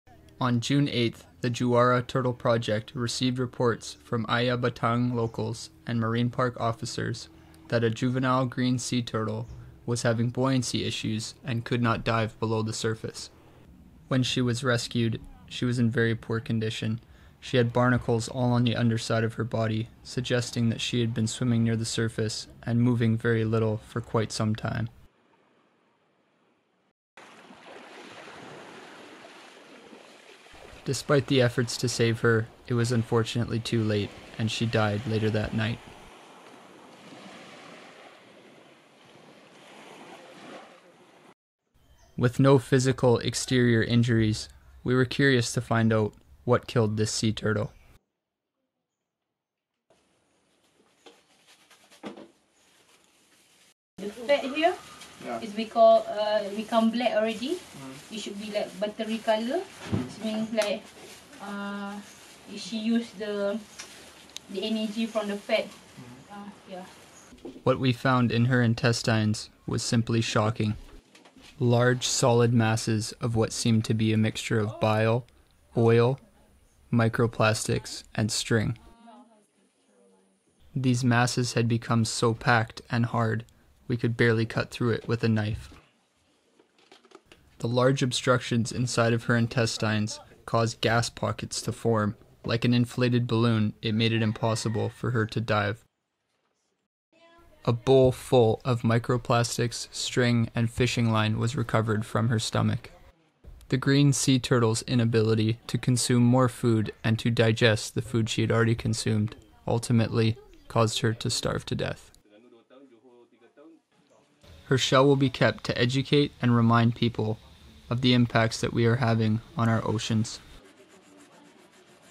Shocking Green Sea Turtle Autopsy
You won't believe what we found inside this turtle! See first hand the impact that humans are having on our oceans.